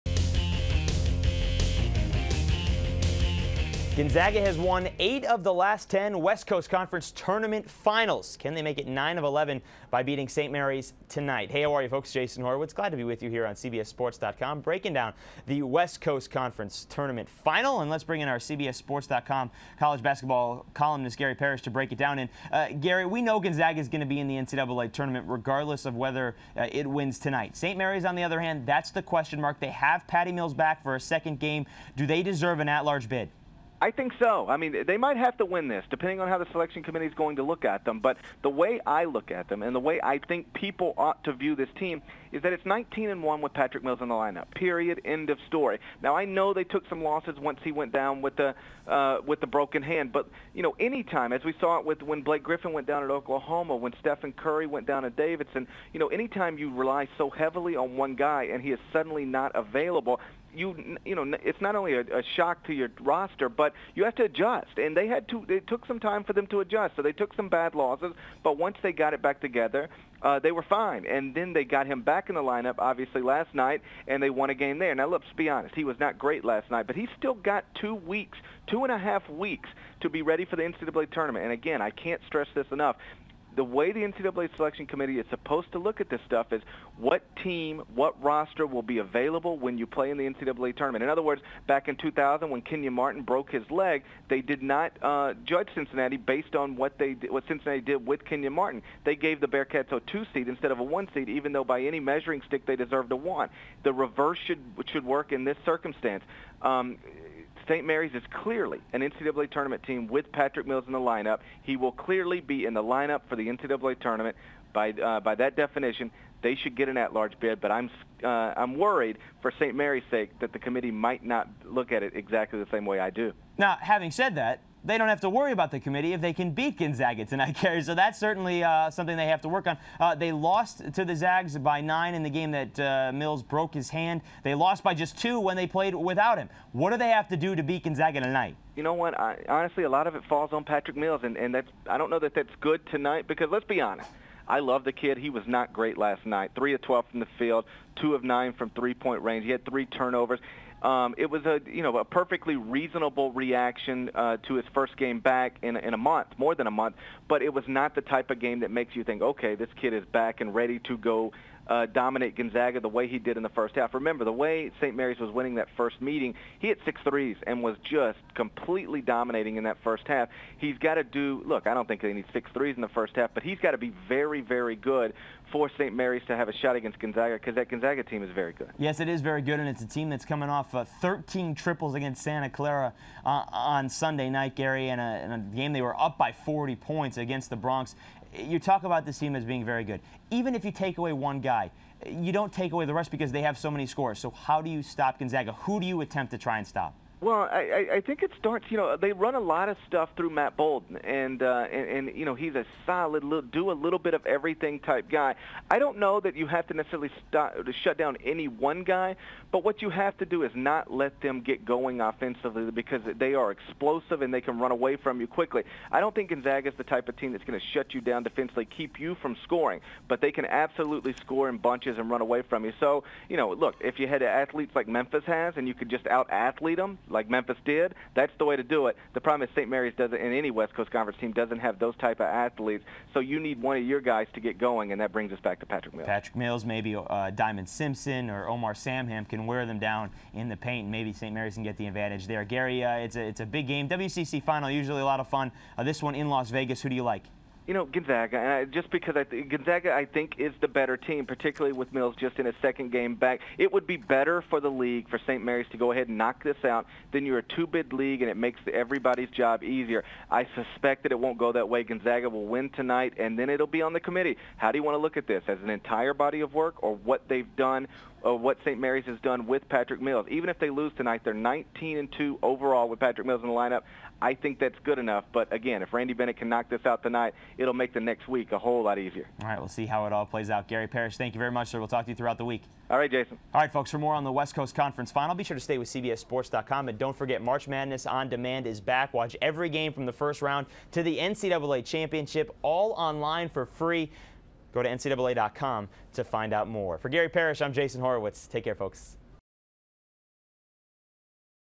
Gonzaga vs St. Mary's - WCC Finals
Gonzaga has won 8 of the last 10 WCC tournaments, but can St. Mary's win knock the Bulldogs off Monday and gurantee a spot in the NCAA tournament. Gary Parrish joins Jason Horowitz to break it down.